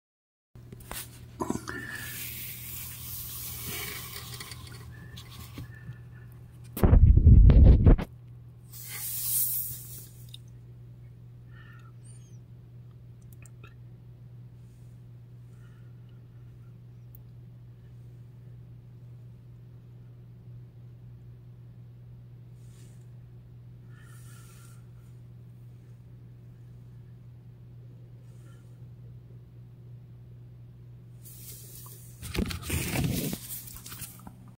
Sears Kenmore Washing Machine-- Pump Motor Shaft Seal Leak
Almost 22 years old. developed a leak during draining phase of wash cycle. Took the base plate off and the location of the leak was obvious. Amazon sells the shaft seal for $4.50. Wholesale parts stores asking $24.00 to $64.00 for a 5mm piece of rubber...SMH.